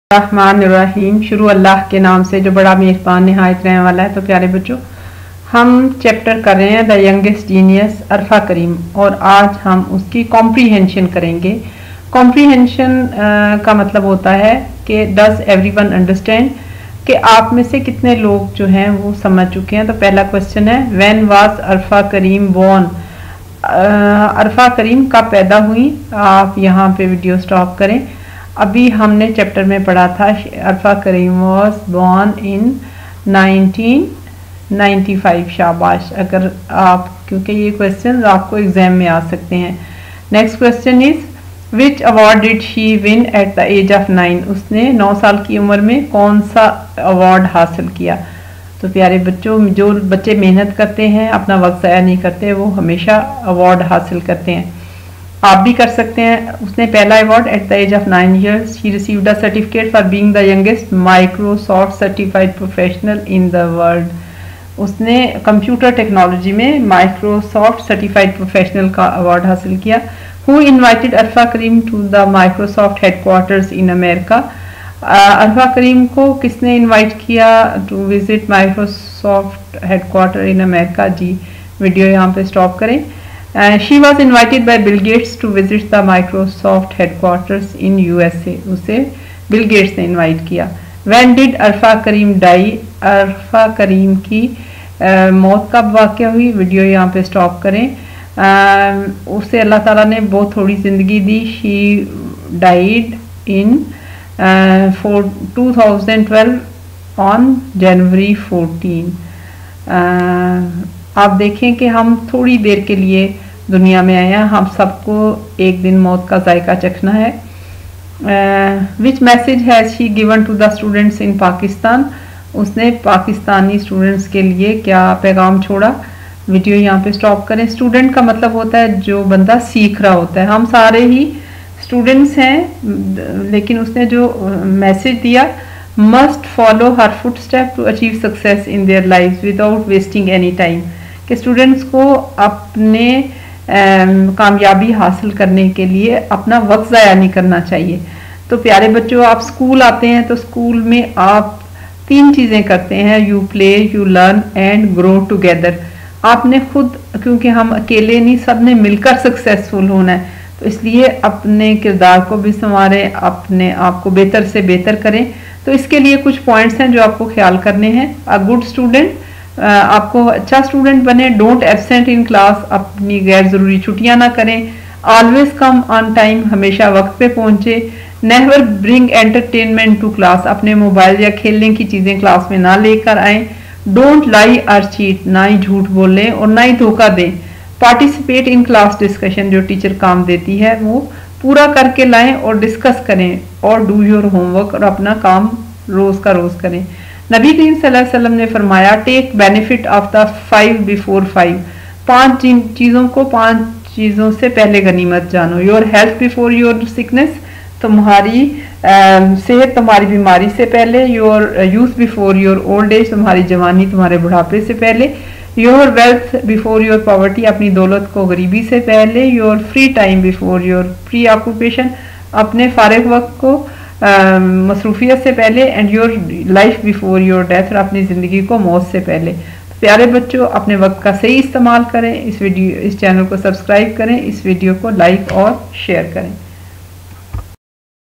The Youngest Genius Arfa Karim/English in Urdu
Comprehension of story The youngest Genius Arfa Karim for grade 5 kids in urdu